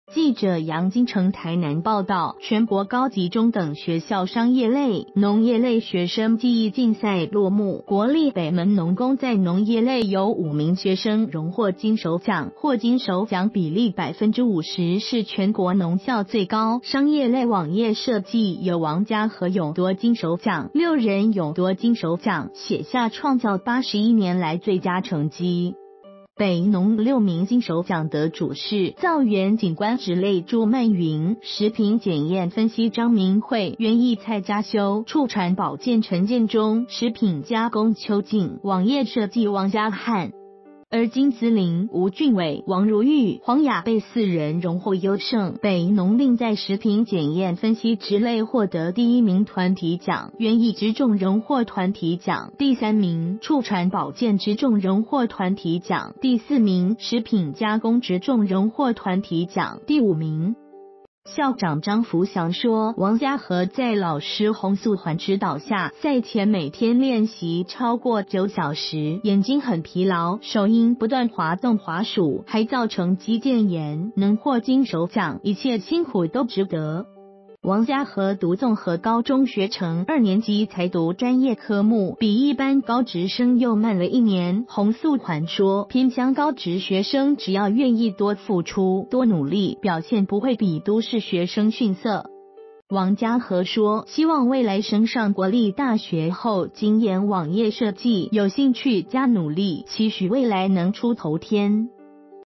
北門農工6生獲金手獎 寫下創校81年最佳成績
〔記者楊金城／台南報導〕全國高級中等學校商業類、農業類學生技藝競賽落幕，國立北門農工在農業類有5名學生榮獲金手獎，獲金手獎比例50％是全國農校最高，商業類網頁設計有王家和勇奪金手獎，6人勇奪金手獎，寫下創校81年來最佳成績。

發表評論評論如下。